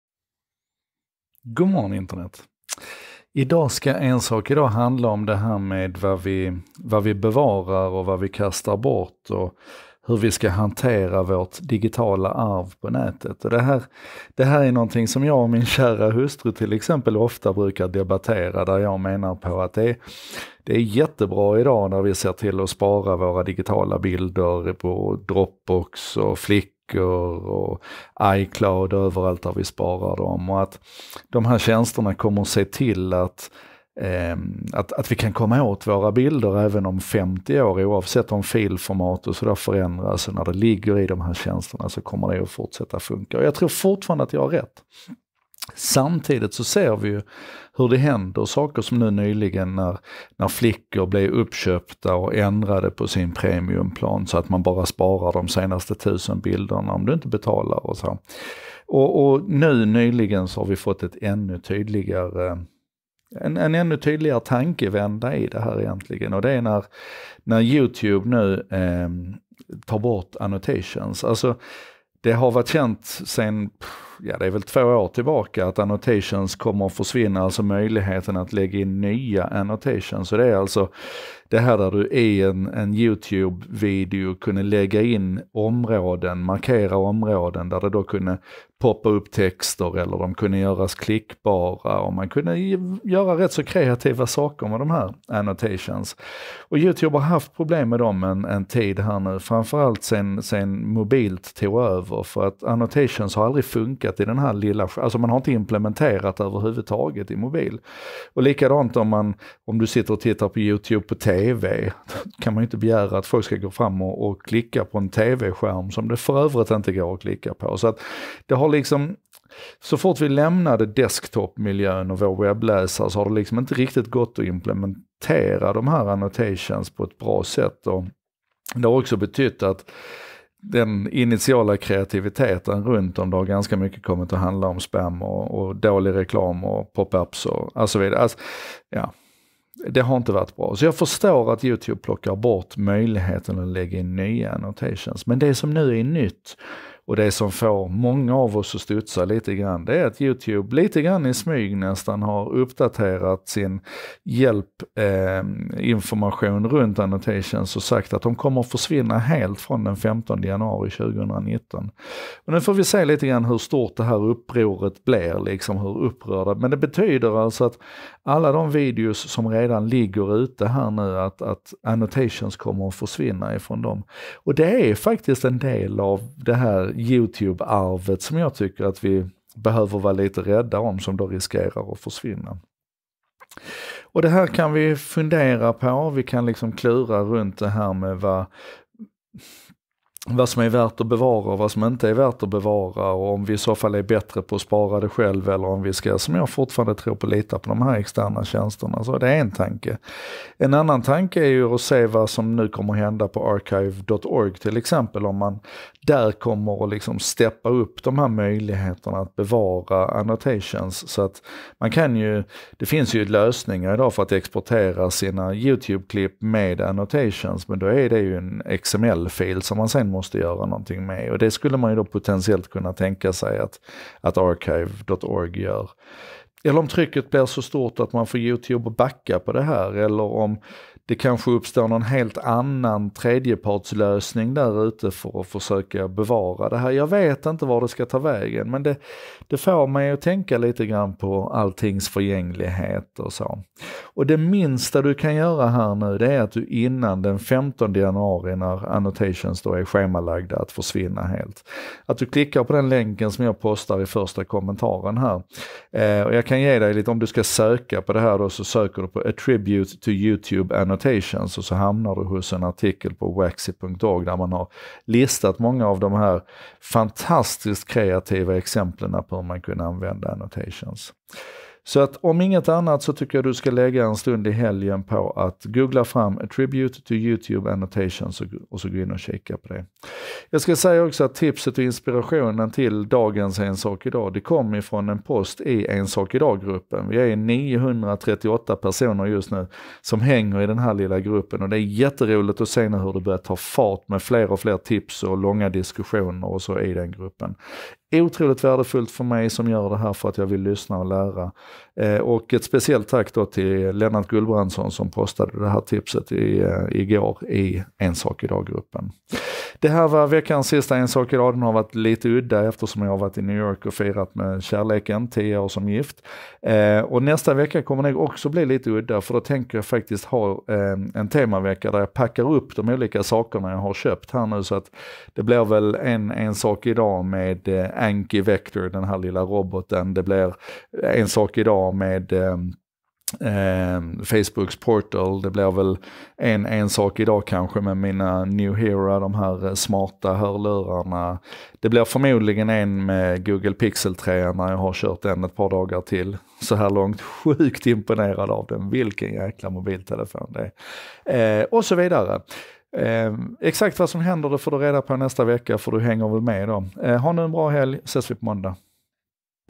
275. Youtube dödar annotations och det är läge att funderar över alltings förgänglighet
275. Youtube dödar annotations och det är läge att funderar över alltings förgänglighet
Beslutet att plocka bort möjligheten att skapa annotations kändes ganska odramatiskt. Beslutet att inte längre visa de som redan finns är avsevärt mer dramatiskt. Det är 10 års kreativitet som nu riskerar att försvinna. Du har fram till den 15 januari på dig att njuta - om inte något något händer…